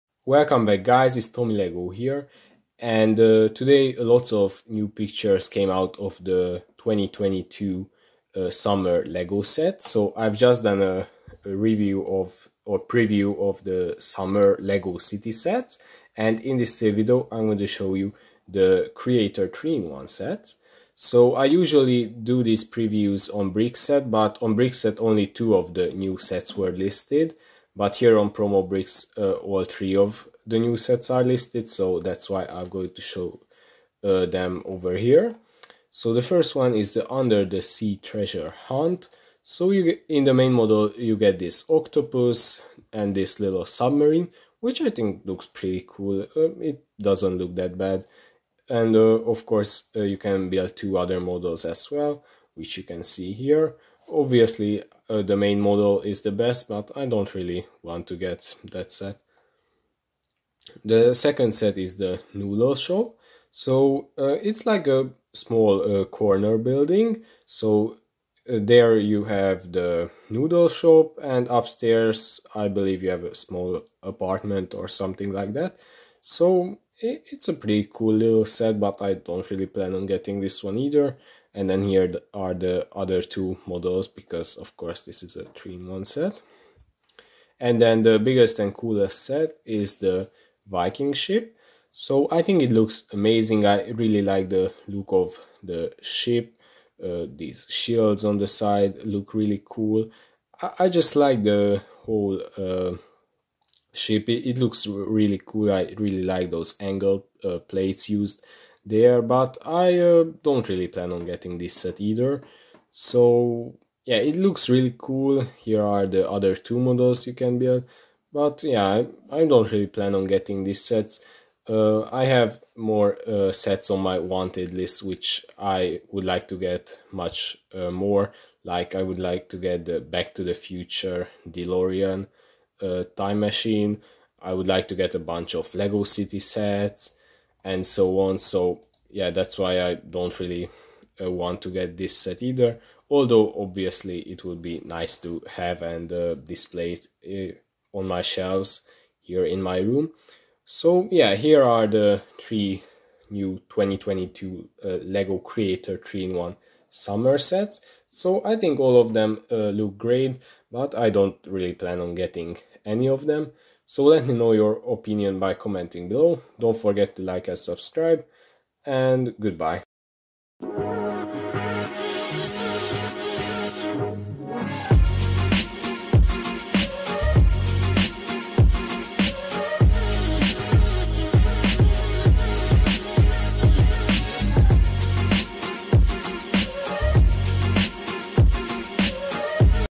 NEW 2022 LEGO CREATOR 3IN1 SUMMER SETS PREVIEW!
In this video I am going to show you some pictures of the new 2022 LEGO Creator 3in1 summer sets. I think they all look great, but I don't really plan on getting any of them.

00:00 Preview
02:50 Subscribe